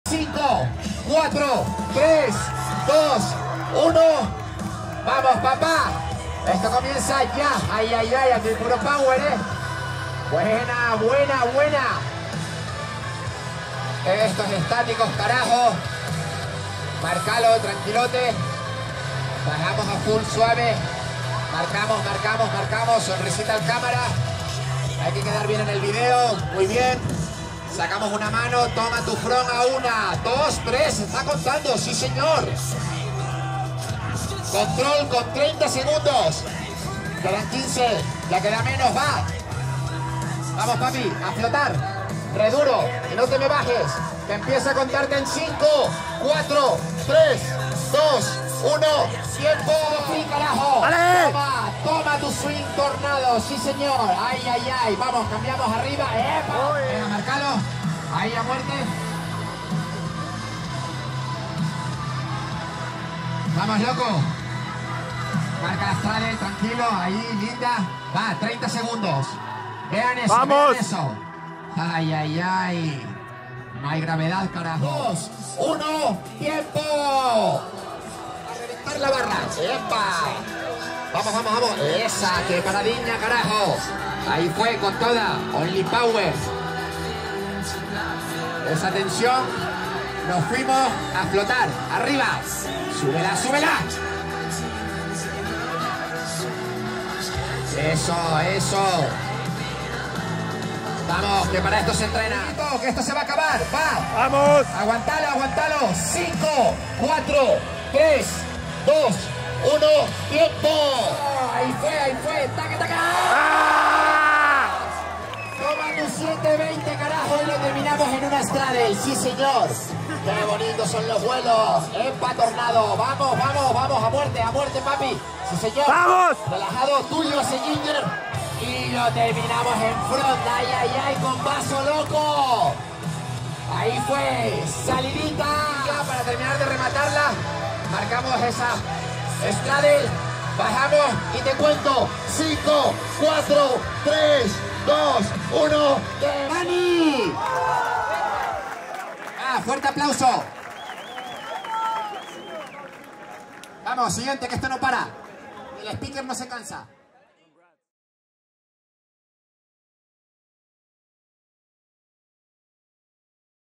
Barmania Battles 2024 | Daniel VS Pol Codina
Timecodes:
0:00 - Pol round 1
0:50 - Daniel round 1
1:19 - Pol round 2
1:51 - Daniel round 2
2:26 - And the winner is…